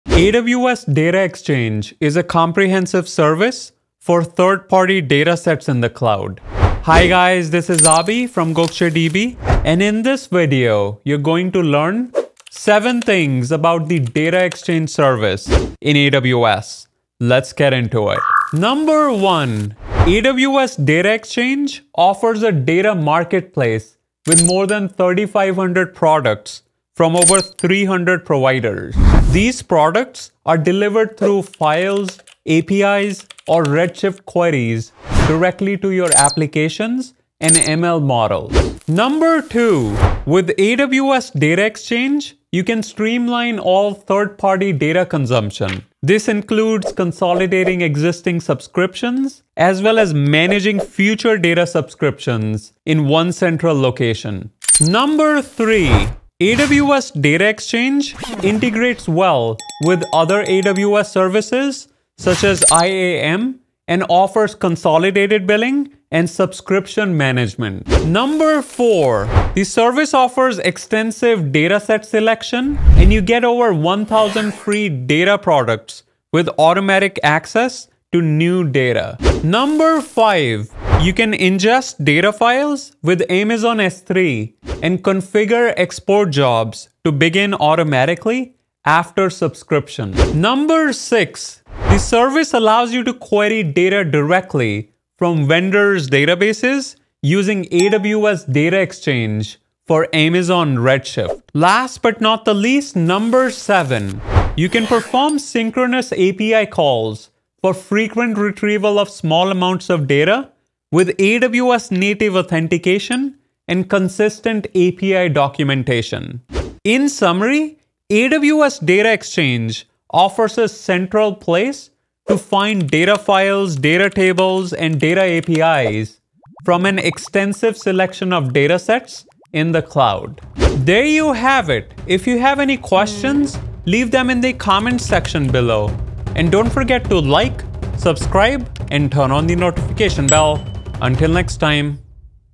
AWS Data Exchange: 7 Things You HAVE To Know ⚡️
AWS Data Exchange is a comprehensive service for third-party data sets in the cloud.

Hi Guys, this is Abi from GokceDB and in this video, you are going to learn 7 things about the Data Exchange service in AWS. Let's get into it.

1. AWS Data Exchange offers a data marketplace with more than 3500 products from over 300 providers. These products are delivered through files, APIs, or Redshift queries directly to your applications and ML models.

2. With AWS Data Exchange, you can streamline all third-party data consumption. This includes consolidating existing subscriptions as well as managing future data subscriptions in one central location.

3. AWS Data Exchange integrates well with other AWS services such as IAM and offers consolidated billing and subscription management.

4. The service offers extensive Data Set selection and you get over 1000 free data products with automatic access to new data.

5. You can ingest data files with Amazon S3 and configure export jobs to begin automatically after subscription.

6. The service allows you to query data directly from vendor's databases using AWS Data Exchange for Amazon RedShift.

7. You can perform synchronous API calls for frequent retrieval of small amounts of data with AWS-native authentication and consistent API documentation.

In summary, AWS Data Exchange offers a central place to find data files, data tables, and data APIs from an extensive selection of data sets in the cloud.